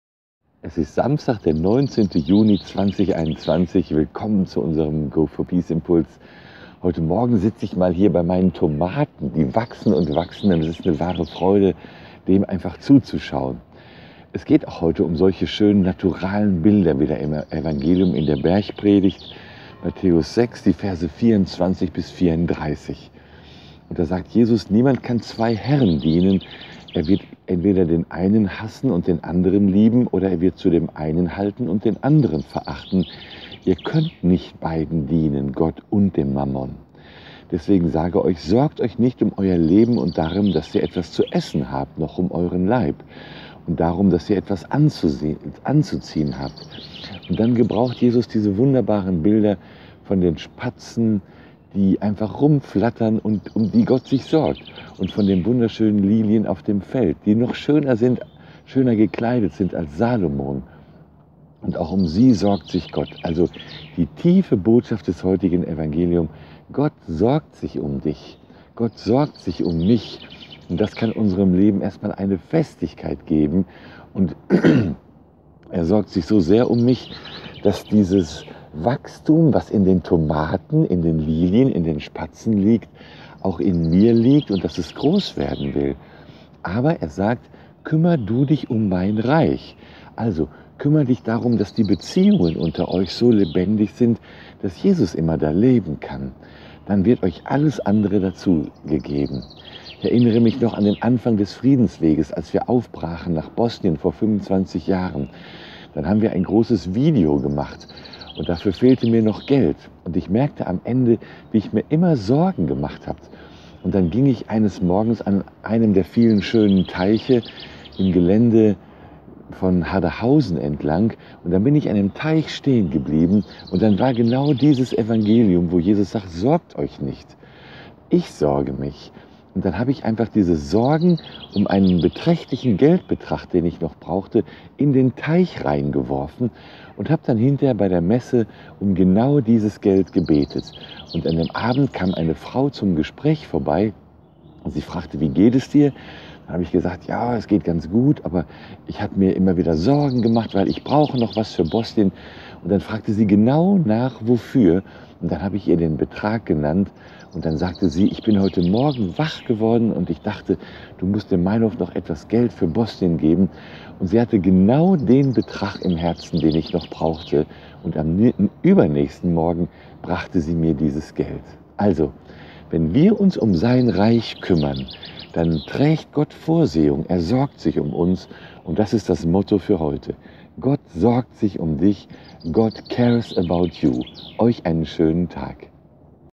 2021 06 19 Gott sorgt sich um Dich!
Motto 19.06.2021: God cares for you! - Mt 6, 24-34 (Do not worry!)

Meinolf: It is Saturday, June 19th, 2021. Welcome to our go4peace impulse. This morning I am sitting here with my tomatoes. They grow and grow. It is a real joy just to watch them. It is about such beautiful natural images again today in the Gospel in the Sermon on the Mount, Matthew 6:24-34. There Jesus says:
“No one can serve two masters. Either you will hate the one and love the other, or you will be devoted to the one and despise the other. You cannot serve both God and money. “Therefore I tell you, do not worry about your life, what you will eat or drink; or about your body, what you will wear." (Mt 6:24-25)
And then Jesus uses these wonderful images of the sparrows just fluttering around and God cares about them, and the beautiful lilies of the field, dressed even more beautifully than (King) Solomon. And God cares for them too.
So the deep message of today's Gospel: God cares about you! God cares for me! That can first of all give our lives a solidity. He cares so much for me that this growth that is in the tomatoes, in the lilies, in the sparrows, is also in me, and that it wants to grow. But he always says: You take care of my kingdom! Take care that the relationships among you are so alive that Jesus can always live there, then everything else will be added to you.
I still remember the beginning of the peace journey when we left for Bosnia 25 years ago. Then we made a big video, but I didn't have the money for that. I realized at the end how I was always worried. Then one morning I was walking along one of the many beautiful ponds in the grounds of Hardehausen. Then I stopped at the pond. Then there was this very Gospel where Jesus says: "Do not worry! I am worrying. - Then I simply threw this worry into the pond for a considerable amount of money that I still needed. Afterwards, at Mass, I prayed for that very money. That evening, a woman came by for a chat. I asked, "How are you?" Then I said, "Yes, I'm doing quite well. I was always worried because I still needed something for Bosnia."  Then she asked me exactly: "What for?" And then I told her the amount. - Then she said: "I woke up this morning and I thought, you have to give some more money to Bosnia. She had exactly the amount in her heart that I still needed. The next morning she brought me this money.

So, if we take care of his kingdom, then God has providence. He cares for us, and that is the motto for today. "God cares for you! Have a nice day!"

For German transscription see facebook "go4peace in Europe".